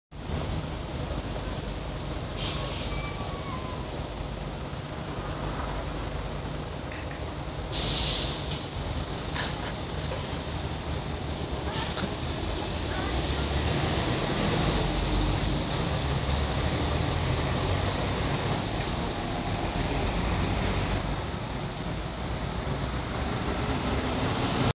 鳥の大群①
突然現れた謎の大群です。 場所は静岡駅前です。